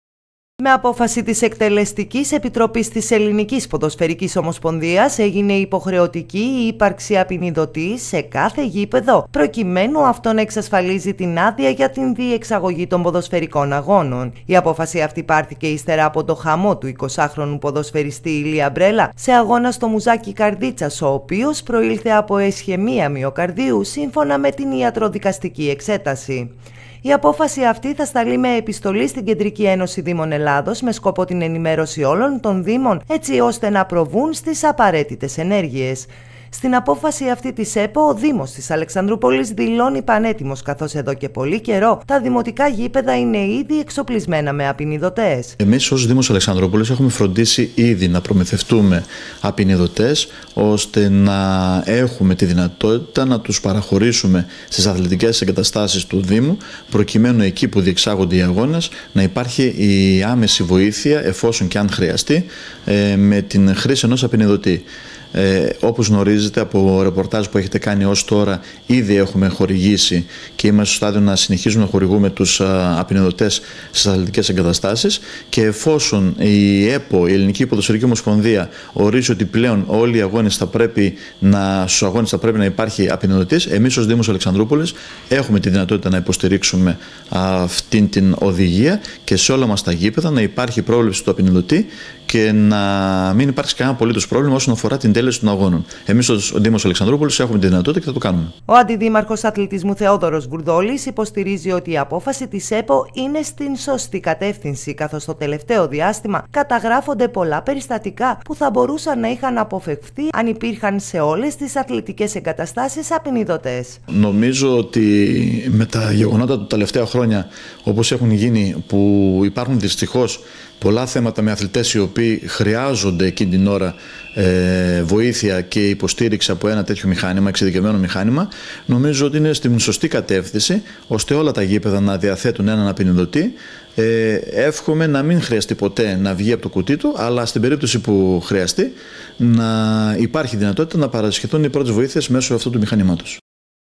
Υποχρεωτική η ύπαρξη απινιδιτών σε όλα τα γήπεδα ποδοσφαίρου
Ο Δήμος της Αλεξανδρούπολης είναι πανέτοιμος να εφαρμόσει την απόφαση της ΕΠΟ για τους απινιδωτές. Πρόκειται για μία απόφαση της Ελληνικής Ποδοσφαιρικής Ομοσπονδίας η οποία θα σταλεί με επιστολή στην Κεντρική Ένωση Δήμων Ελλάδας, με την οποία θα ζητά να ενημερωθούν όλοι η Δήμοι που έχουν στην ιδιοκτησία τους γήπεδα ότι πλέον θα είναι υποχρεωτική η ύπαρξη απινιδωτή ως προϋπόθεση για την διεξαγωγή αγώνων.